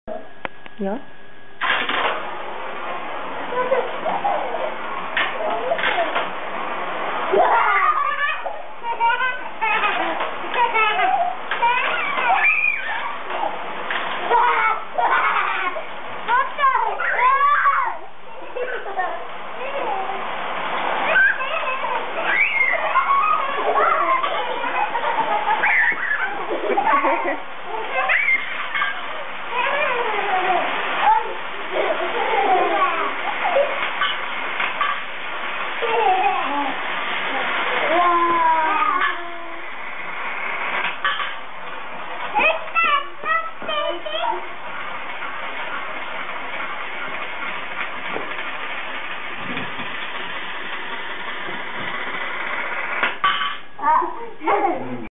Spiral Wishing Well in Japan
This clip shows that laughter and fun is the same in any language.  Spiral Wishing Wells are fun for children of all ages. When it was first posted, the total amount of money raised for charities had reached $200 million...now it is over $1 BILLION and continuing to grow.